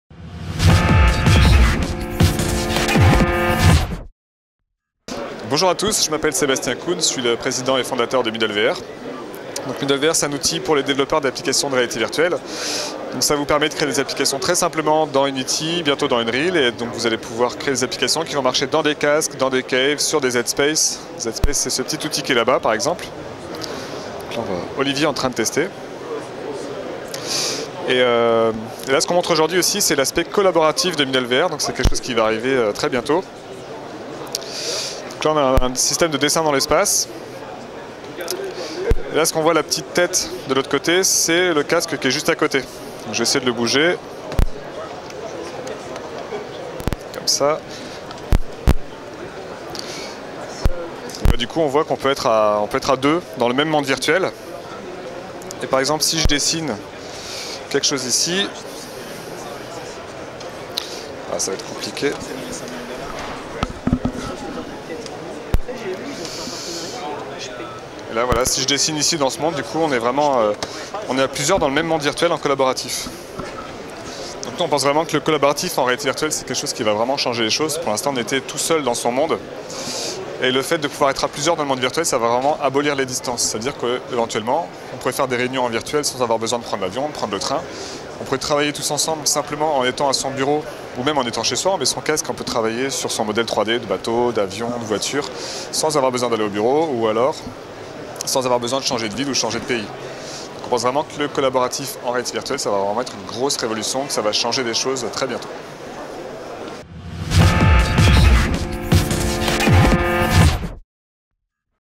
MiddleVR - La VR collaborative par Sébastien Kuntz @ LAVAL VIRTUAL 2015
MiddleVR (previously ” i’m in VR ” ) is a software company providing immersive virtual reality (VR) applications and services.

Their flagship product, MiddleVR SDK, is a VR toolbox to accelerate the creation of VR applications.

MiddleVR SDK is fully integrated into the powerful 3D application creation tool Unity3D. See MiddleVR for Unity.